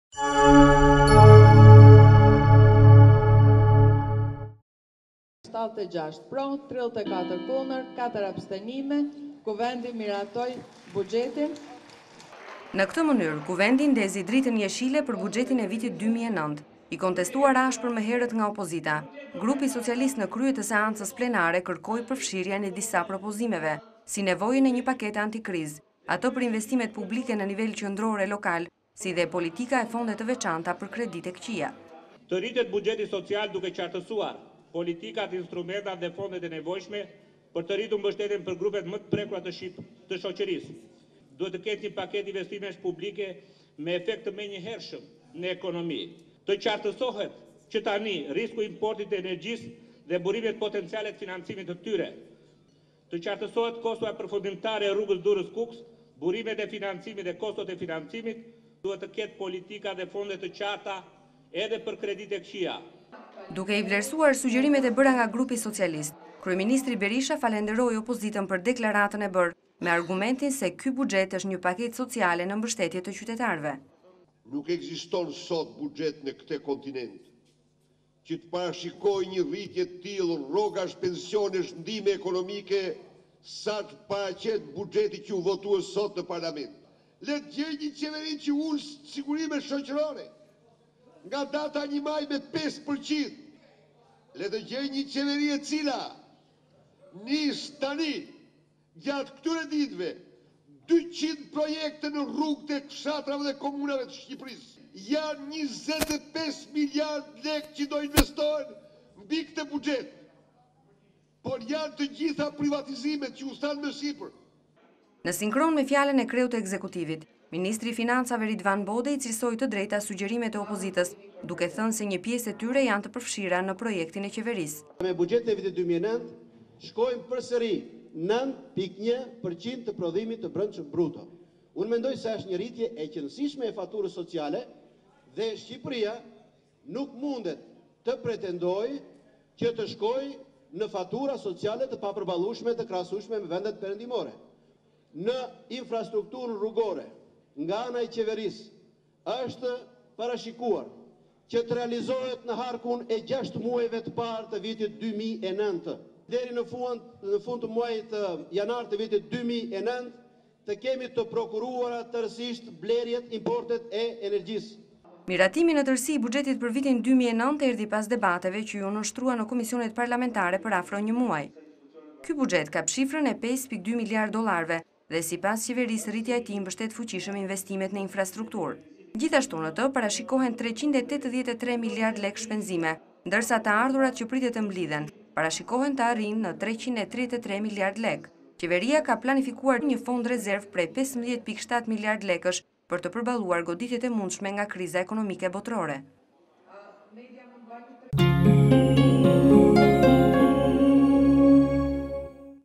Kuvendi miraton buxhetin e vitit 2009, i kontestuar ashpër nga opozita - (27 Nëntor 2008)
Jozefina Topalli: “76 pro, 34 kundër, 4 abstenime. Kuvendi miratojë buxhetin”.

Në ketë mënyre Kuvendi ndezi dritën jeshile për buxhetin e vitit 2009. I kontestuar ashpër më herët nga opozita,  grupi socialist në krye të seancës plenare kërkoi përfshirjen e disa propozimeve, si nevojën e një pakete anti-krizë, atë për investimet publike në nivel qendror e lokal, si dhe politika e fonde të veçanta për kreditë e këqija.

Arben Malaj: “Të rritet buxheti social, duke qartësuar politikat, instrumentat dhe fondet e nevojshme për të rritur mbështetjen për grupet më të prekura të shoqërisë. Duhet të ketë një paketë investimesh publike me efekt të menjëhershëm në ekonomi, të qartësohet që tani risku i importit të energjisë dhe burimet potenciale të financimit të tyre, të qartësohet kostoja përfundimtare e rrugës Durrës-Kukës, burimet e financimit e kostot e financimit. Duhet të ketë politika dhe fonde të qarta edhe për kreditë e këqija”.

Duke i vlerësuar sugjerimet e bëra nga grupi socialist, kryeministri Berisha falënderoi opozitën për deklaratën e bërë me argumentin se ky buxhet është një paketë sociale në mbështetje të qytetarëve.

Sali Berisha: “Nuk ekziston sot buxhet në këtë kontinent që të parashikojë një rritje të tillë rrogash, pensionesh, ndihmë ekonomike saç paraqet buxheti që u votua sot në parlament. Le të gjejnë një qeveri që ul sigurimet shoqërore nga data 1 maj me 5%, le të gjejë një qeveri e cila nis tani gjatë këtyre ditëve 200 projekte në rrugët e fshatrave dhe komunave të Shqipërisë. Janë 25 miliard lekë që do investohen mbi këtë buxhet, por janë të gjitha privatizimet që u thanë më sipër”.

Në sinkron me fjalën e kreut të ekzekutivit Ministri i Financave Ritvan Bode, i cilësoi të drejta sugjerimet e opozitës duke thënë se një pjesë e tyre janë të përfshira në projektin e qeverisë.

Ridvan Bode: “Me buxhetin e vitit 2009, shkojnë përsëri 9.1% të prodhimit të brendshëm bruto. Unë mendoj se është një rritje e qenësishme e faturës sociale dhe Shqipëria nuk mundet të pretendojë që të shkojë në fatura sociale të pa përballueshme e të krahasueshme në vendet perëndimore. Në infrastrukturën rrugore nga ana e qeverisë, është parashikuar që të realizohet në harkun e 6 muajve të parë të vitit 2009. Deri në fund të muajit janar të vitit 2009 të kemi të prokuruar tërësisht blerjet importet e regjisë”.

Miratimi në tërësi i buxhetit për vitin 2009 erdhi pas debateve që iu nënshtruar në komisionet parlamentare për afro një muaj. Ky buxhet kap shifrën e 5.2 miliardë dollarëve dhe sipas qeverise rritja e tij mbështet fuqishëm investimet në infrastrukturë, gjithashtu në të parashikon 383 miliardë lekë shpenzime, ndërsa të ardhurat që pritet të mblidhen parashikohen të arrijnë në 333 miliardë lekë. Qeveria ka planifikuar gjithashtu një fond rezervë prej 15.7 miliardë lekësh për të përballuar goditjet e mundshme nga kriza ekonomike botërore./tvklan.al

Gazetare: Jolldiz Mitro